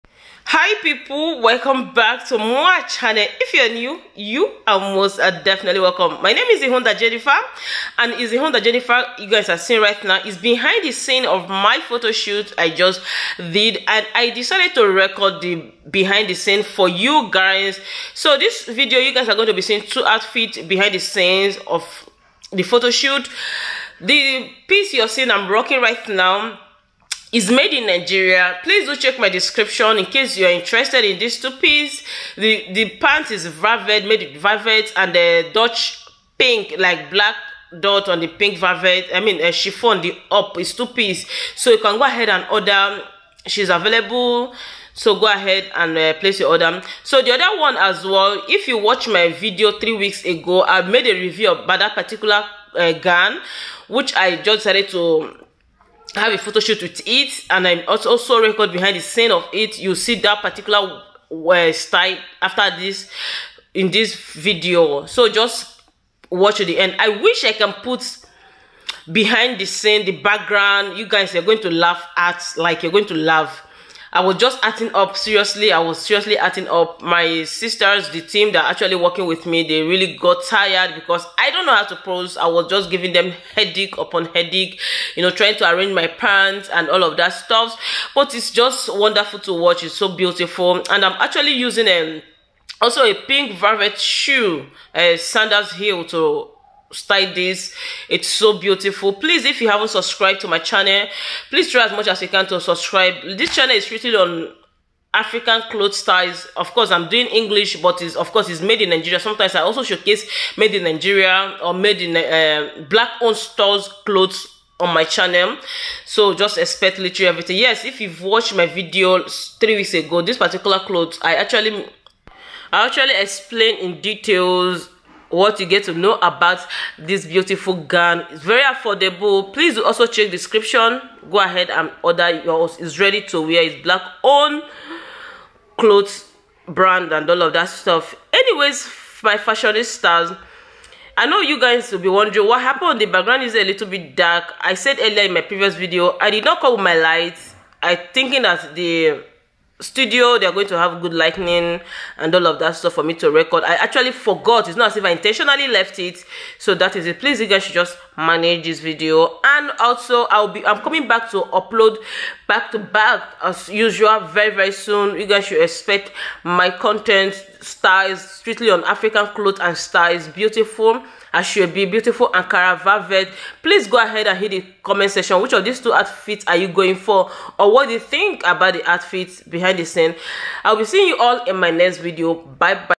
Ihunda Jennifer styles behind the scene look 2 and 3
For the ready to wear  bubu Gowns

his video will show you how to look great in Ankara Styles And Lace Styles,  This African Fashion  is unique and excellent. Designer sometimes you use net material or other fabrics,